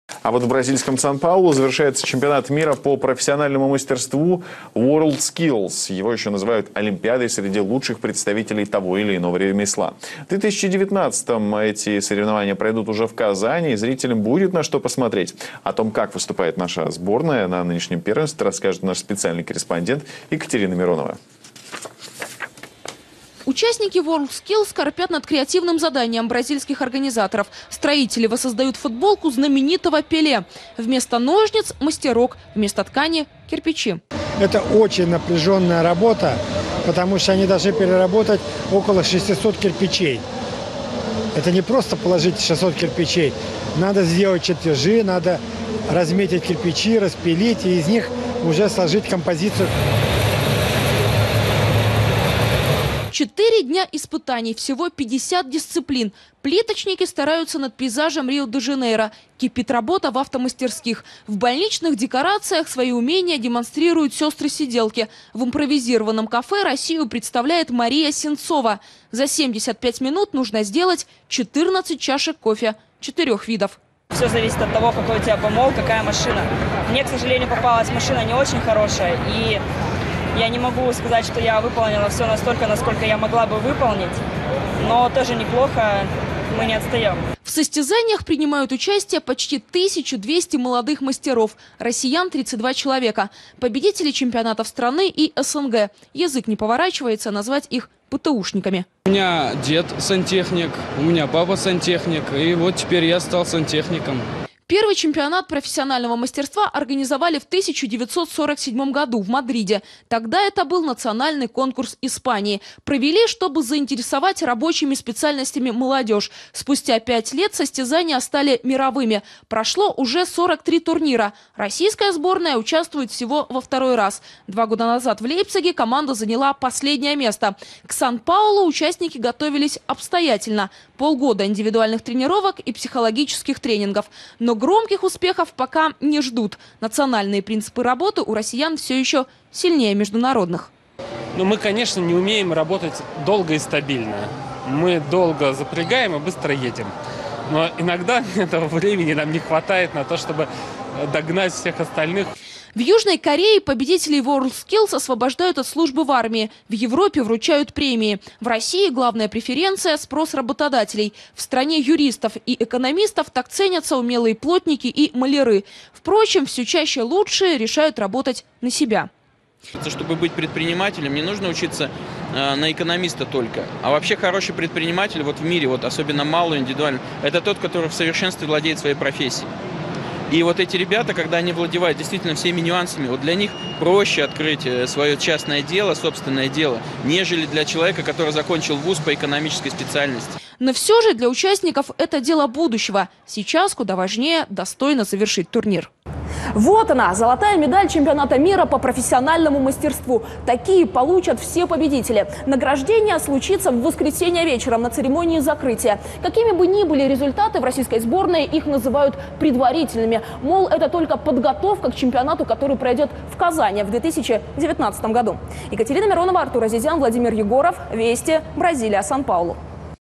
В бразильском Сан-Паулу завершается чемпионат мира WSC-2015
В бразильском Сан-Паулу завершается чемпионат мира по профессиональному мастерству WorldSkills. (Россия 24)